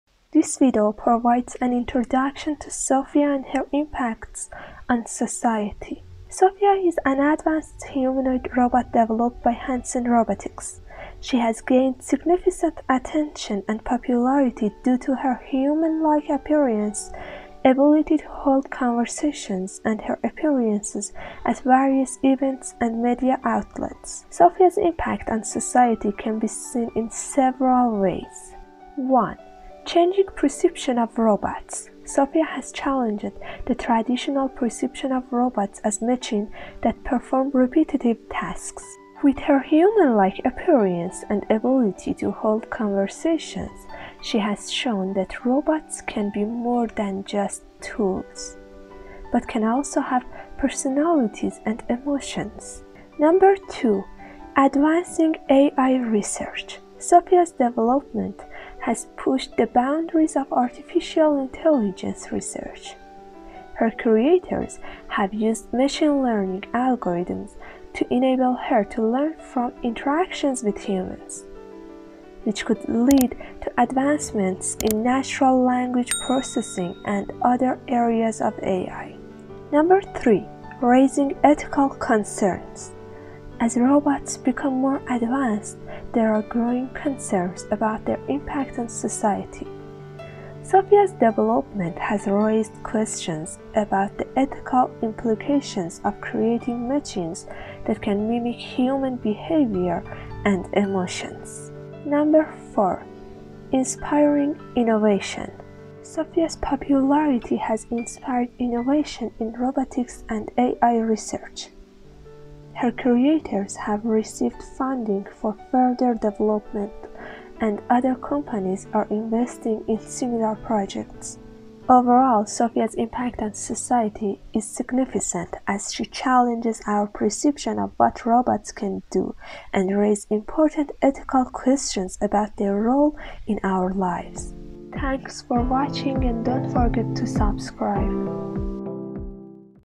Force of Sophia: How One Woman's Impact Changed the World
Sophia is an AI-powered robot created by Hanson Robotics. Sophia has advanced facial recognition and speech recognition capabilities, allowing her to interact with people in a natural way. Sophia Sensors are a suite of sensors designed to enable Sophia to interact with her environment and sense the presence of people and objects around her. The sensors include cameras, microphones, infrared sensors, and tactile sensors.